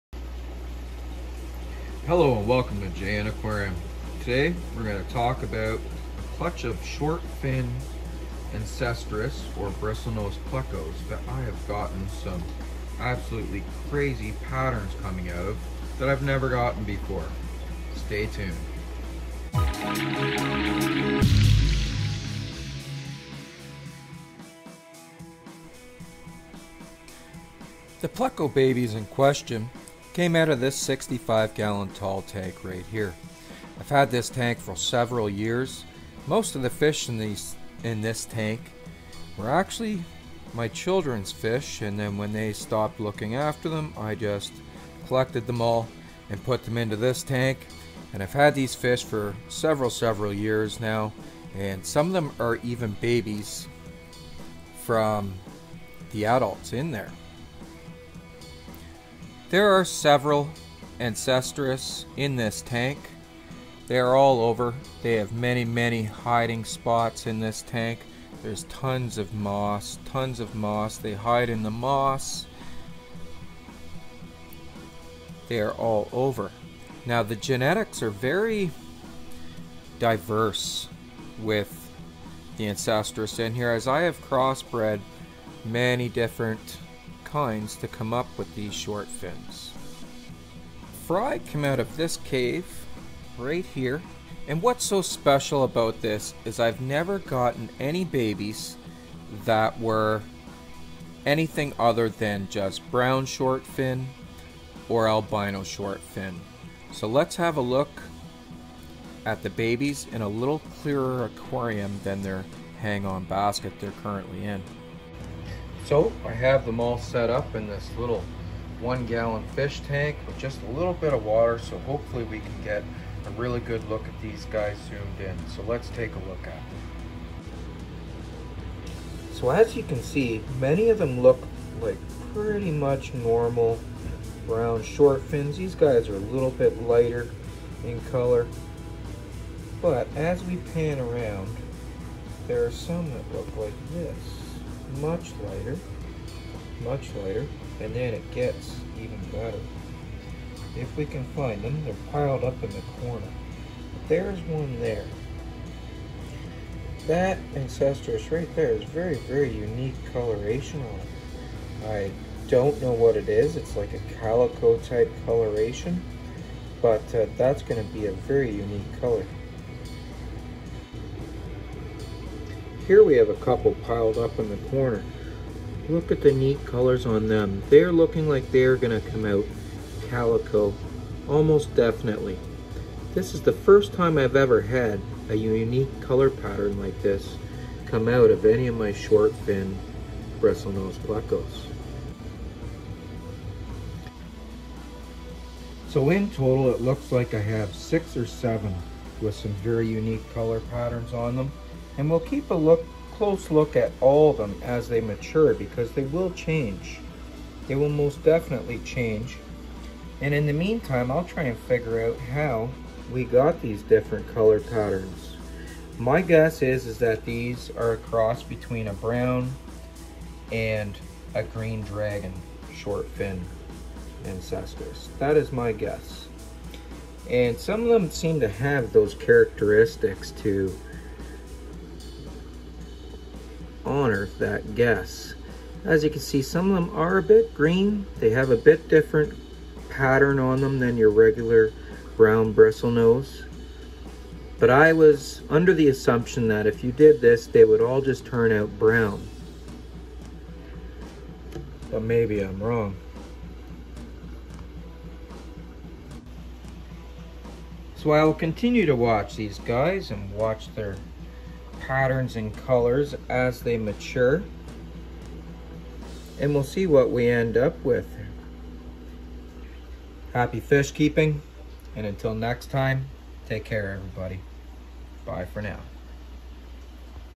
Bristle nose pleco fry Weird patterns and colours
I noticed some unusual colours and patterns in some recent ancistrus fry.
  We will take a closer look at them and try and figure out how they came about!